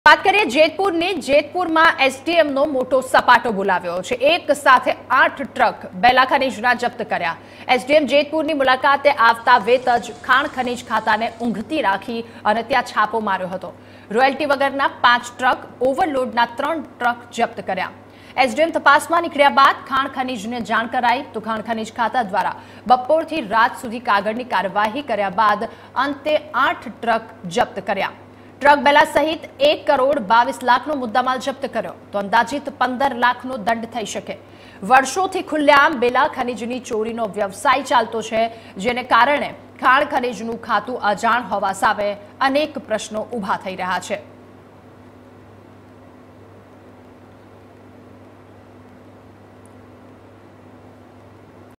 SDM In Jetpur : જેતપુરમાં SDMનો સપાટો - Latest Gujarat News
જેતપુરમાં SDMનો સપાટો બોલાવ્યો છે, એક સાથે 8 ટ્રક બેલા ખનીજના જપ્ત કત્ય હતા. SDM જેતપુરની મુલાકાતે આવતા વેંત જ ખાણ ખનીજ ખાતાને ઉંઘતી રાખી છાપો માર્યો હતો. રોયલ્ટી વગરના 5 ટ્રક અને ઓવર લોડના 3 ટ્રક જપ્ત કર્યા હતા. SDM તપાસમાં નીકળ્યા બાદ ખાણ ખાણીજને જાણ કરી હતી. ખાણ ખનીજ ખાતા દ્વારા બપોરથી રાત સુધી કાગળની કાર્યવાહી કર્યા બાદ અંતે 8 ટ્રક કર્યા જપ્ત હતા. ટ્રક બેલા સહિત 1 કરોડ 22 લાખનો મુદ્દામાલ જપ્ત કર્યો હતો. અંદાજીત 15 લાખનો દંડ થઈ શકે છે. વર્ષોથી ખુલ્લેઆમ બેલા ખનીજની ચોરીનો વ્યવસાય ચાલતો હોય અને ખાણ ખનીજ ખાતું અજાણ હોવા સામે પ્રશ્ન ઉભો થયો છે ?